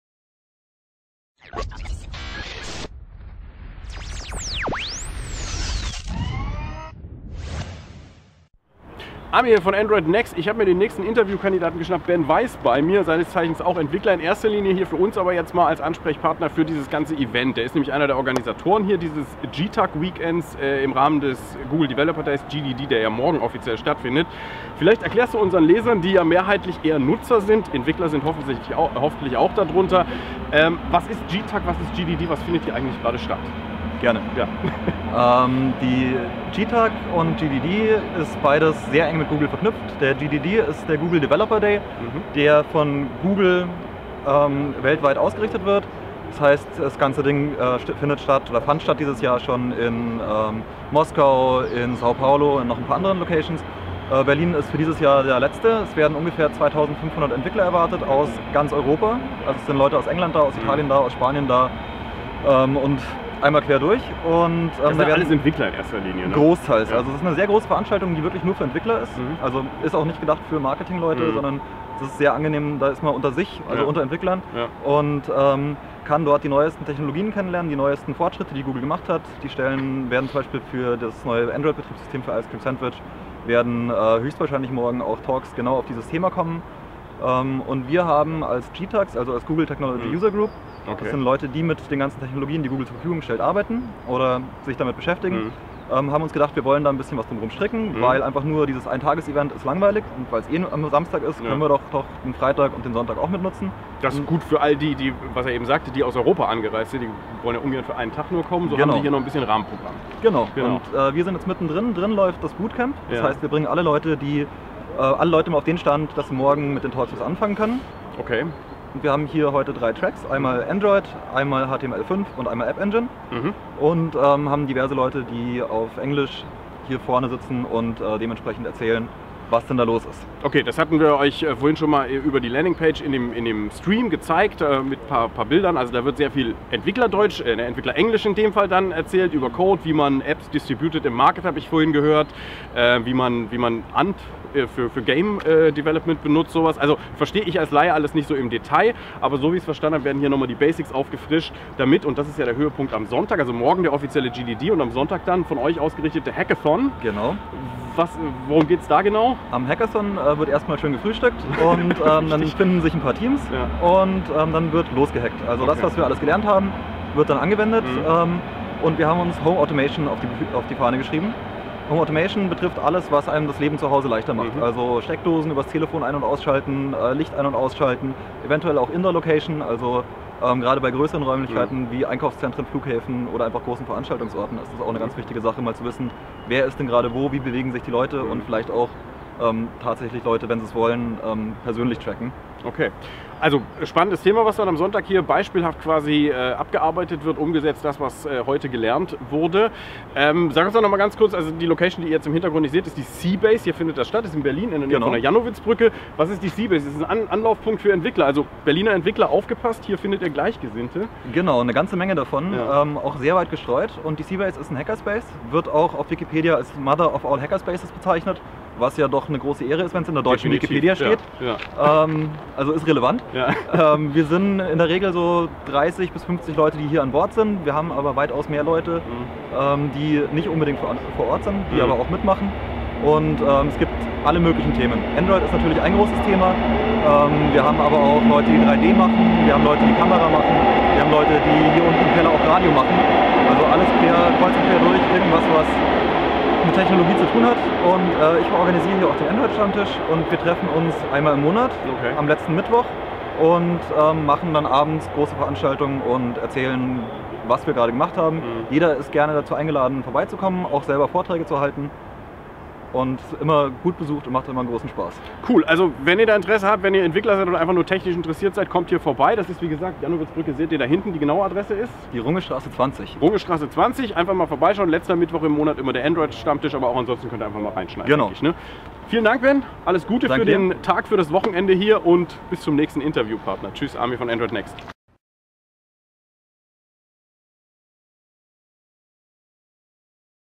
GTUG Berlin GDD 2011 - Interview Ben Weiss - androidnext.de
- Ben Weiss, einer der Mitorganisatoren des GTUG-Weekends zum GDD 2011 Berlin in der c-base, erzählt Amir im Interview, was sich hinter der Google Technology User Group und dem Google Developer Day 2011 verbirgt.